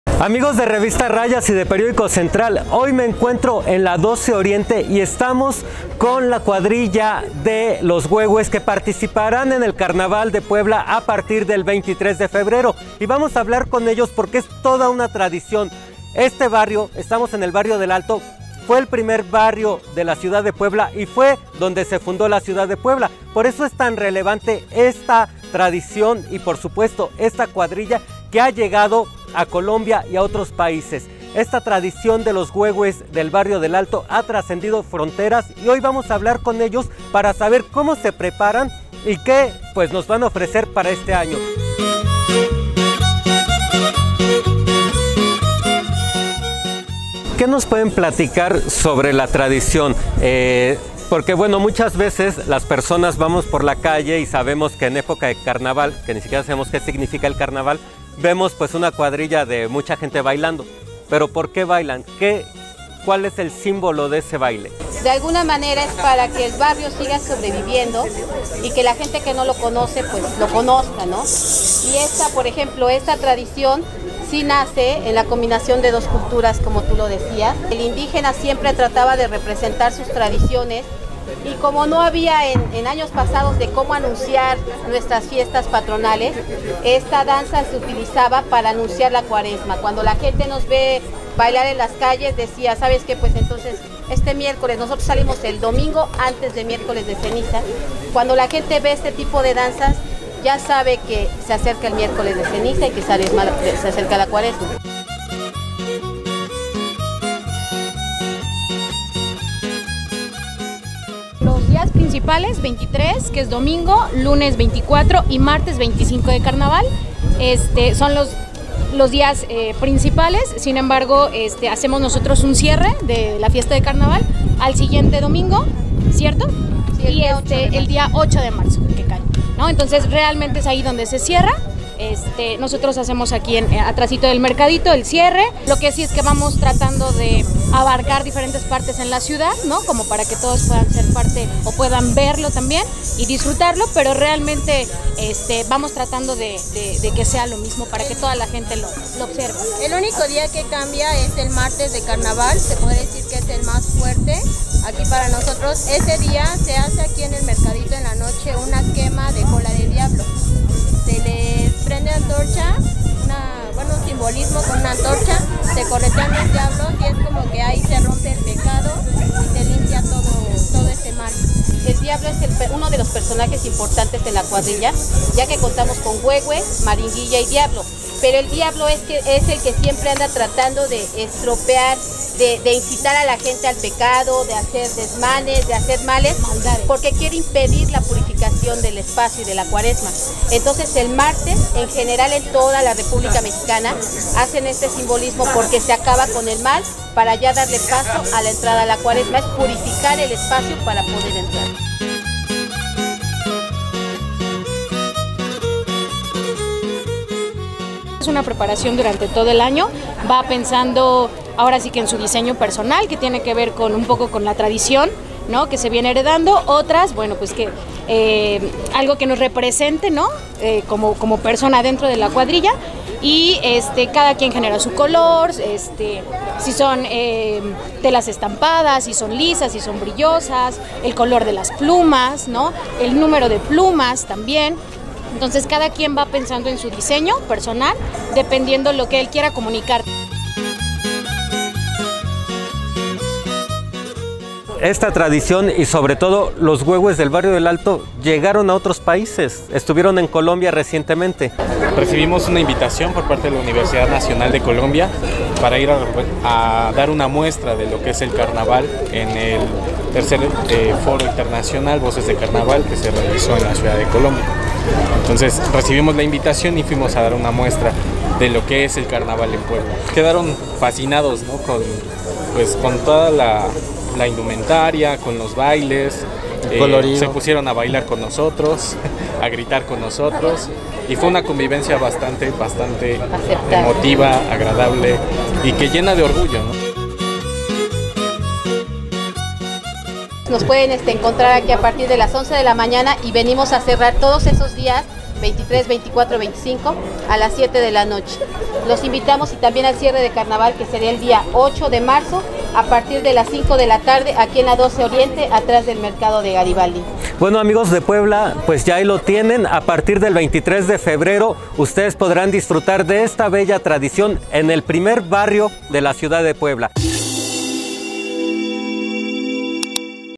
Los Huehues de “El Alto”, la tradición más antigua de Puebla.
Los Huehues del Barrio de Alto integran la cuadrilla más grande en la capital poblana, pues suman 150 personas y como a los niños también les gusta participar, les crearon su propio grupo llamado “Los Garibabys”, que debutarán en este 2020. Pero más allá del número de personas, lo importante es que cada uno de ellos es feliz siendo huehue.